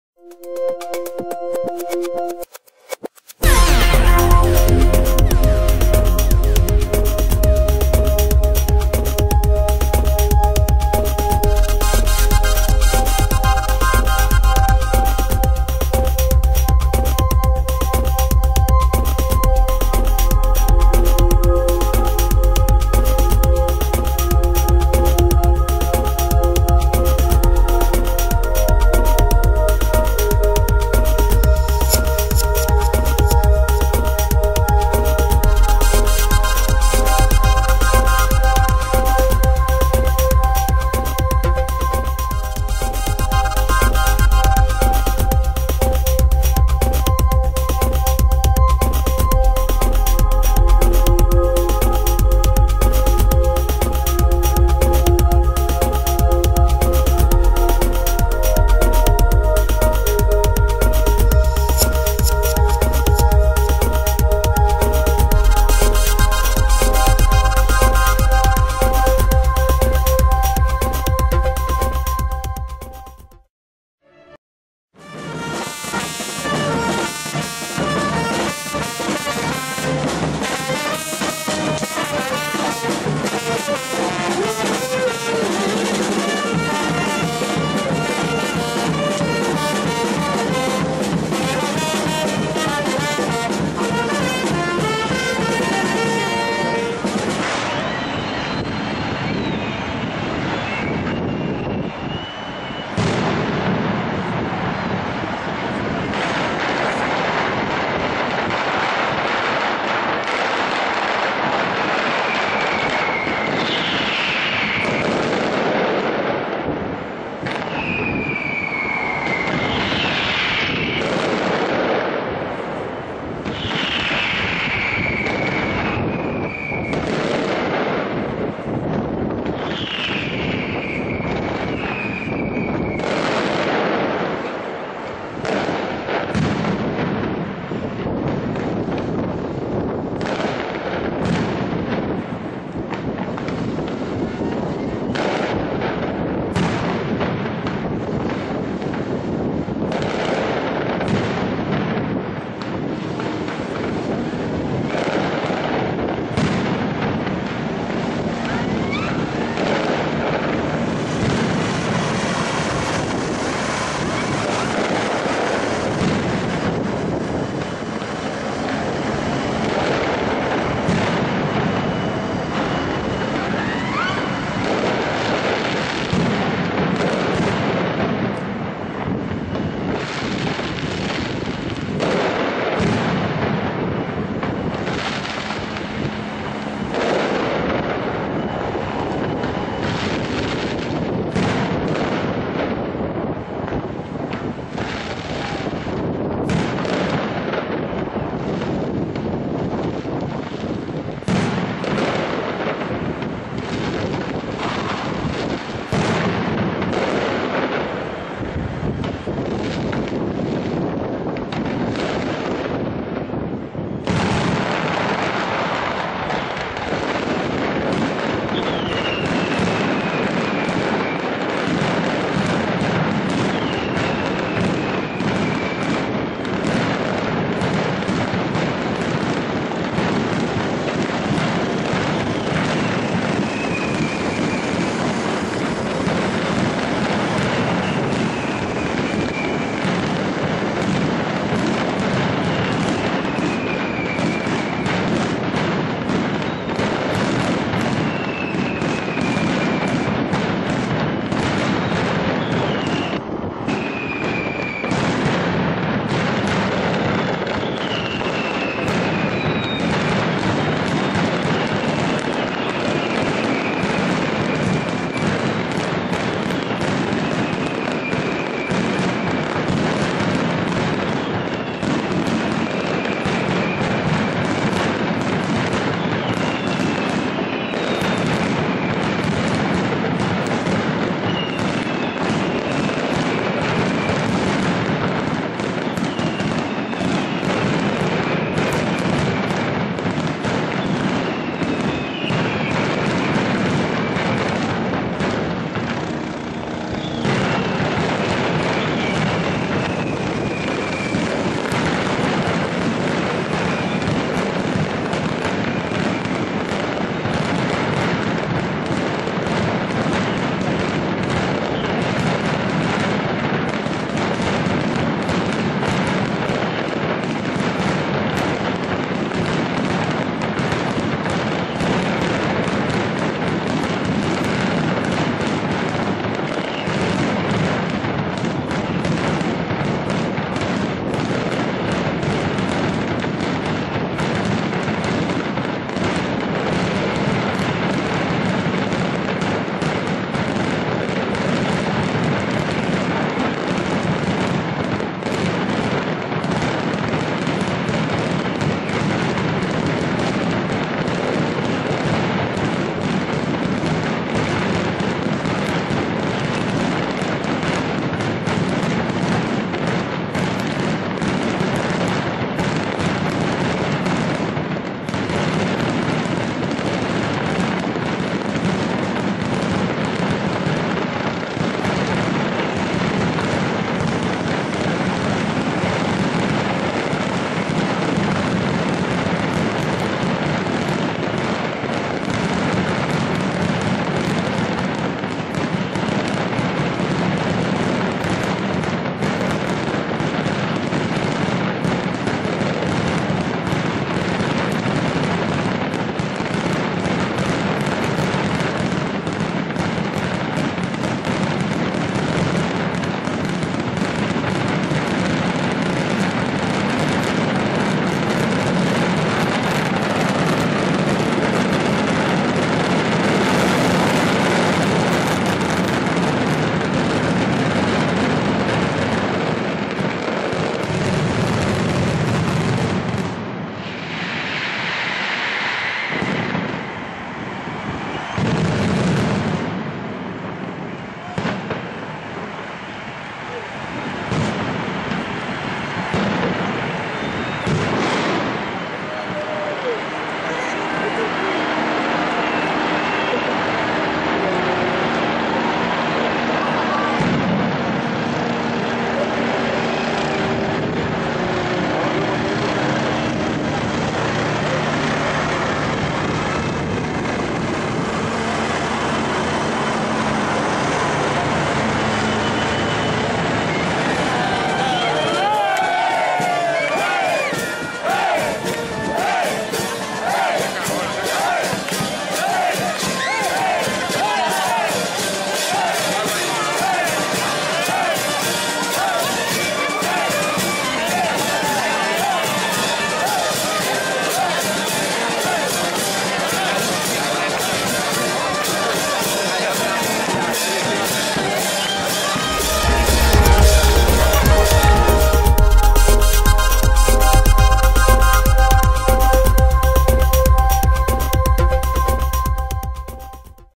Falles de Valencia 2014 - Mascletà Pirotecnica Boreda
Falles de Valencia 2014 - 9 Marzo
Plaza de Ayuntamiento - Mascletà by Pirotecnica Boreda

In collaborazione con GUIDA VALENCIA